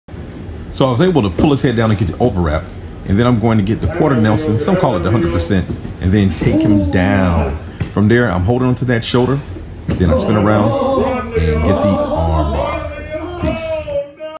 BJJ - Spin Around Arm Bar
- I’ve been working with my grappling dummies a lot as of late. One of the techniques I’ve been practicing is the spin around arm bar. I’m not an arm bar guy by any means, but hard work pays off as the work has been translating to class.